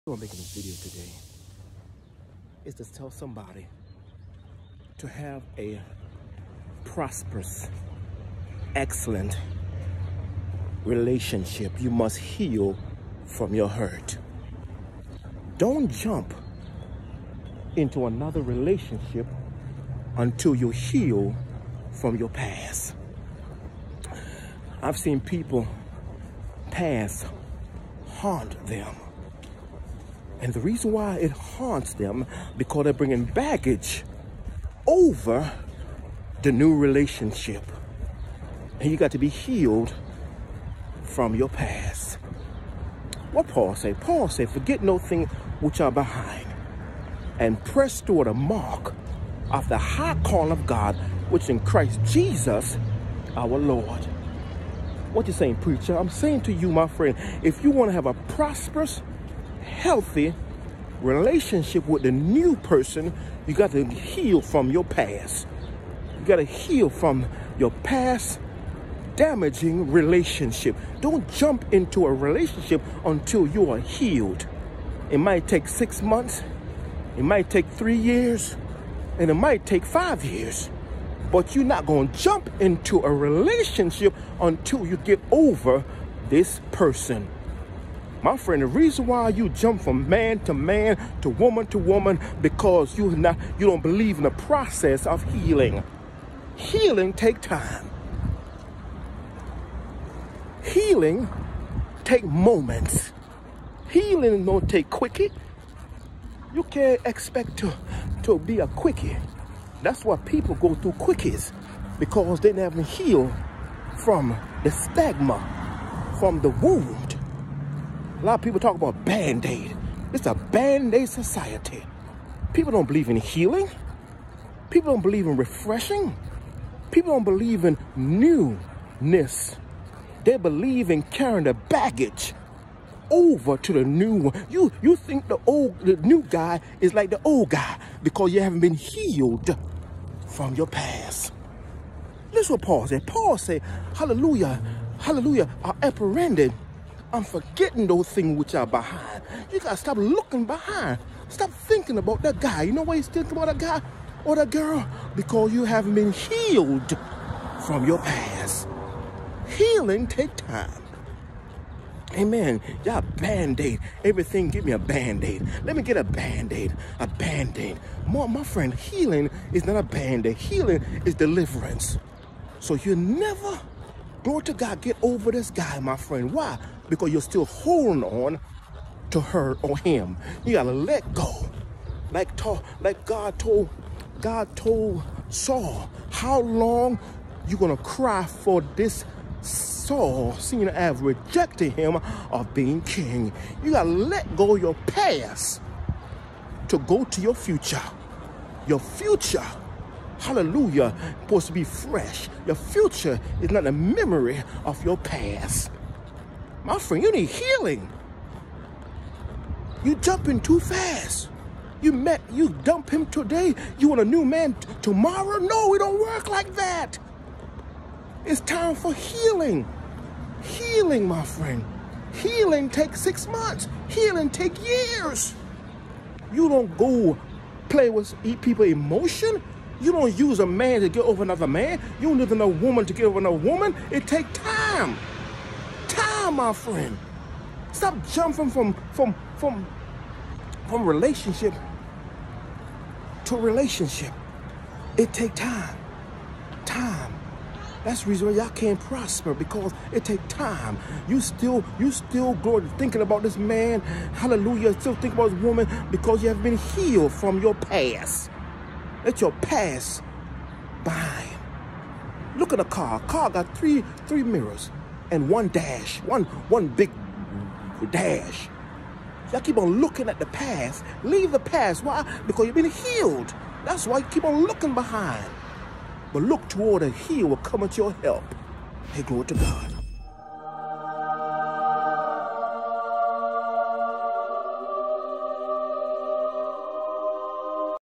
How to be Healed Not Band-Aid from your Past p.1?
We must be heal from our past by being taking time off of dating and seeing someone new.